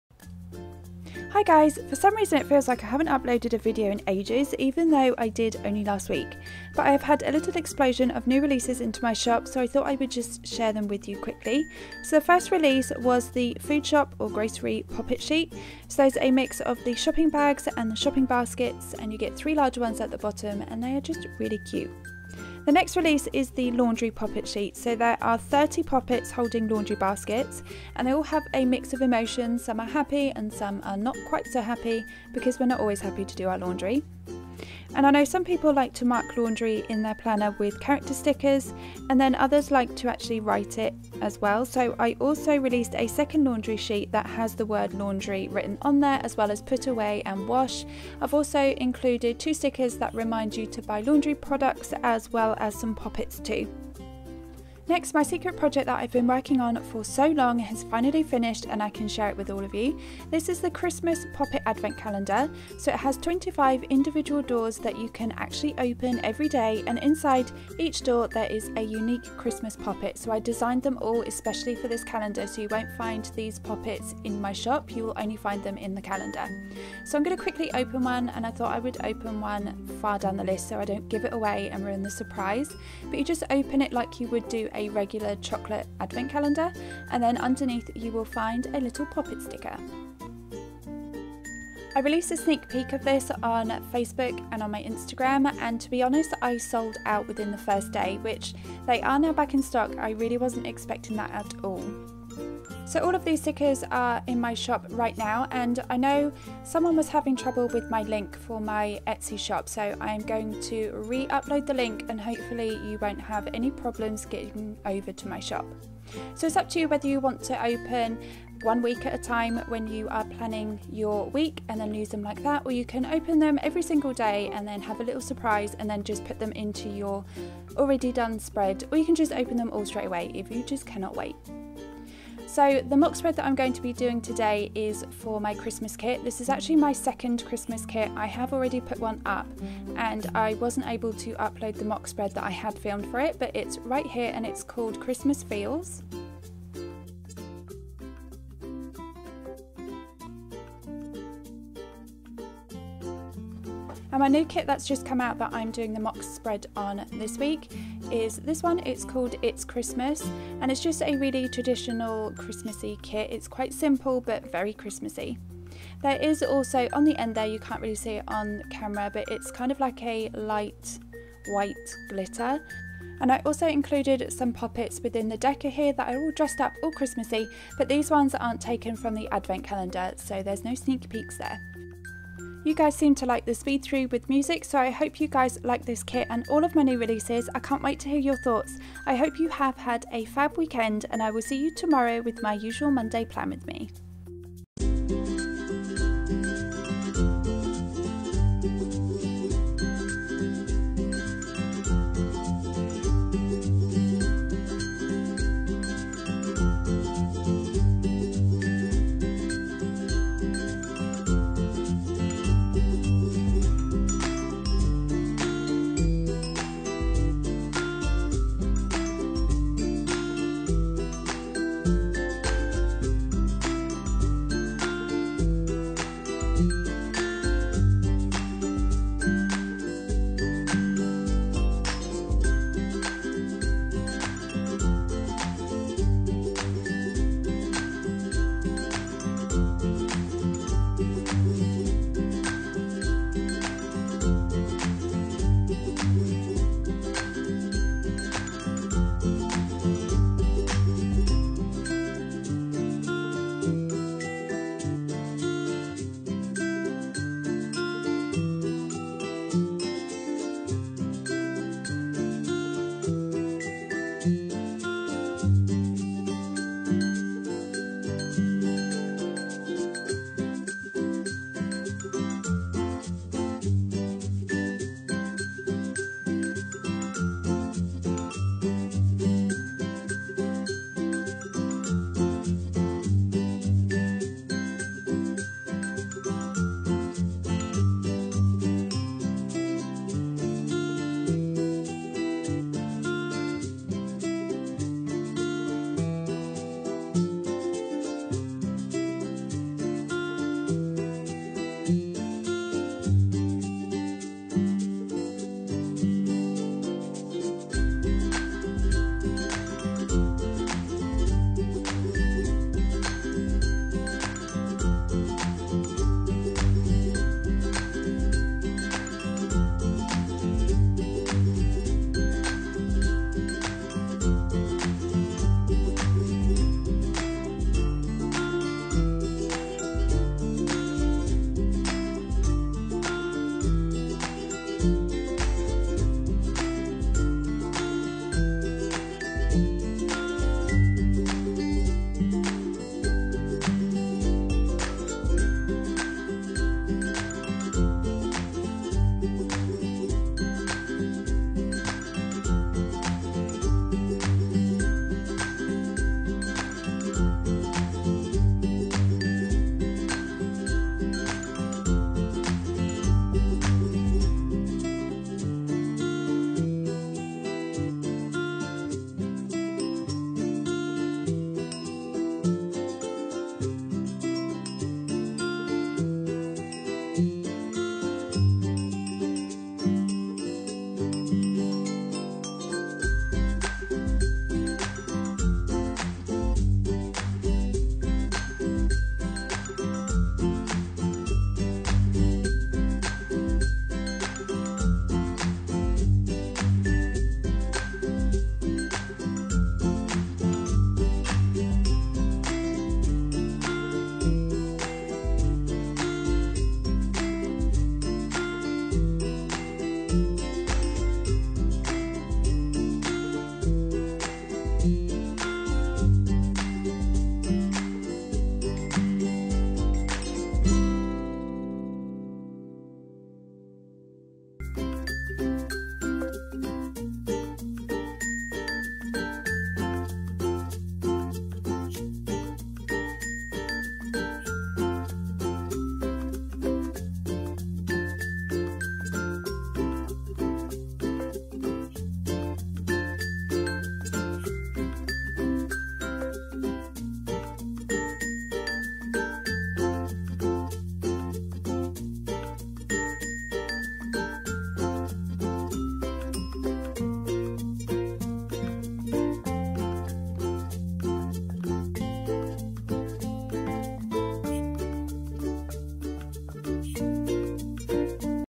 Mock plan with me + secret project reveal!!!!!
Welcome back!

OPEN ME

ETSY SHOP

Late night upload - I have a few new releases in my Etsy shop so I thought I would share them with you before using my newest Christmas kit called - It's Christmas - and I can finally share my secret project I have been working on for so long!! I am so overwhelmed with the positive response I received all of you are amazing and I really want to thank all of you, my shop wouldn't exist without all of your support and it really does mean the world to me to know you are happy with my ideas and designs.

Camera used: Cannon EOS 700d